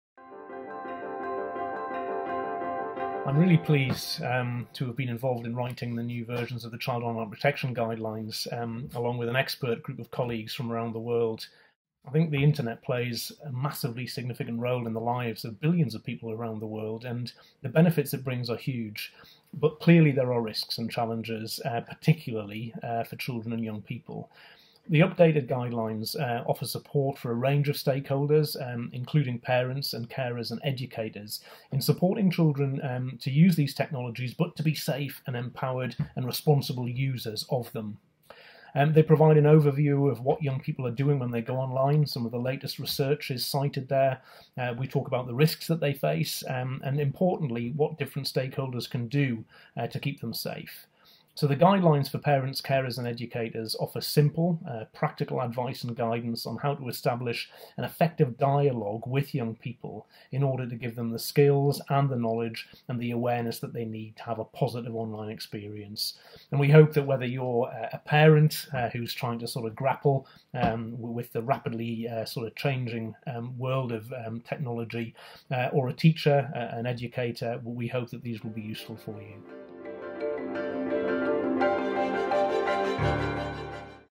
Karl Hopwood on the new 2020 Child Online Protection Guidelines
Video message from Karl Hopwood, independent esafety expert